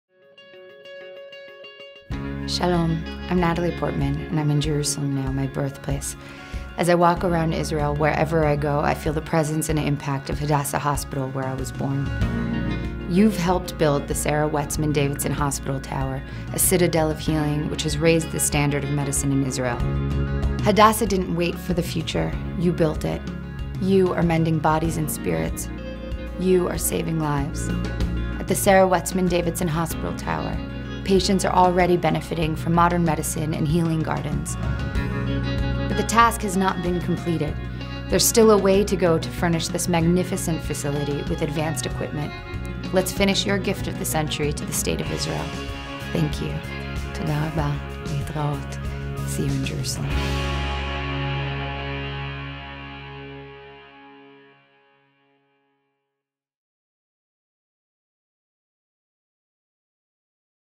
Help Hadassah Heal: Narrated by Natalie Portman
Help Hadassah equip patient rooms in the newly built Sarah Wetsman Davidson Hospital Tower. Miracles happen in these rooms every day, through cutting-edge research and compassionate care. We need to make sure that this vital work continues.